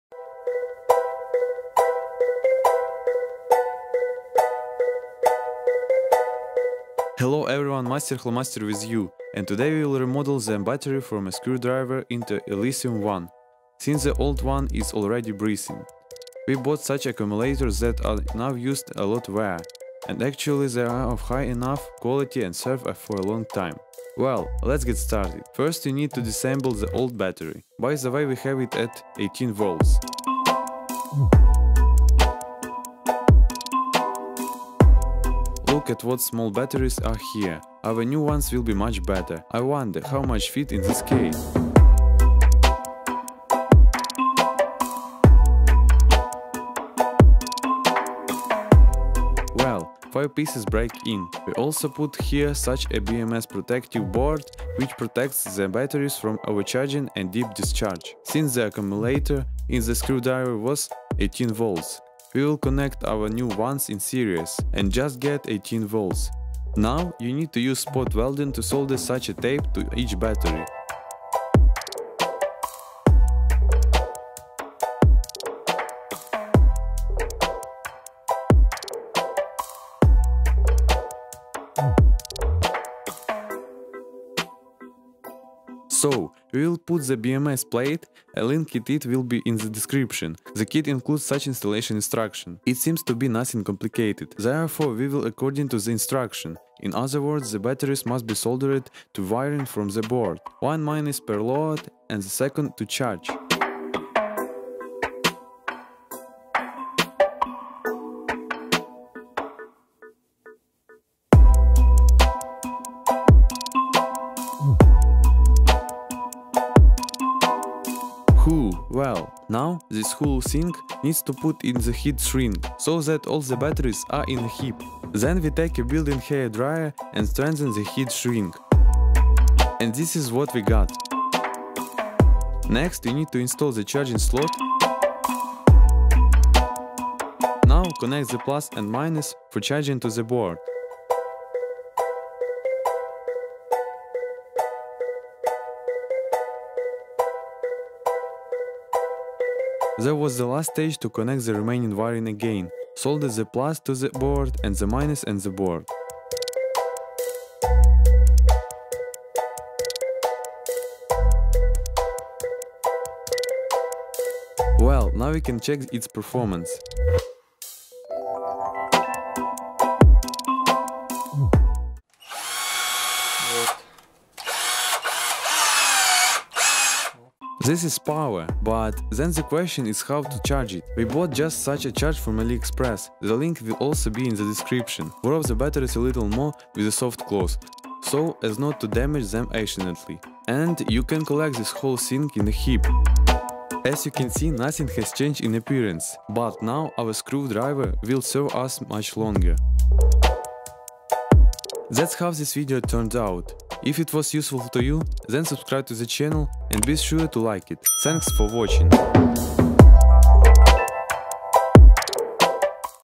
alteration of a screwdriver at 18650
In this video I'll transform this old, cordless power drill Ni-Cd battery to lithium battery by using 18650 Sony VTC5 cells.

Update! From the date when video was made pased some time and Sony released new version of high drain cells with bigger capacity 3120mAh - VTC6. Genuine Sony VTC6 cells could  be bought here: